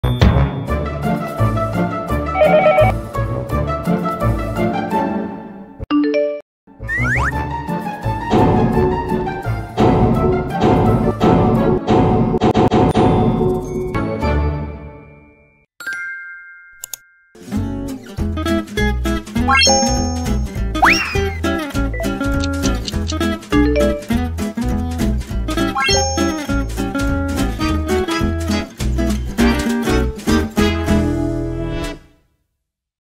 【UNV- Link Pro App】Manage Projects Effortlessly
Calling all installers! Say hello to your new best friend for on-site configuration and remote maintenance - UNV-Link Pro App!

Here's why you'll love this App:

1️⃣ Simplify on-site configuration with easy steps - no more headaches over complicated setups!
2️⃣ Manage projects and devices effortlessly by configuring devices in batches and delivering them to users hassle-free.
3️⃣ Conduct remote maintenance like a pro! Configure user-entrusted devices remotely or log in to the device web with ease.
4️⃣ Diagnose issues in a snap with remote packet capture capabilities.

Ready to take your installation game to the next level? Try UNV-Link Pro today and experience efficiency like never before! 💼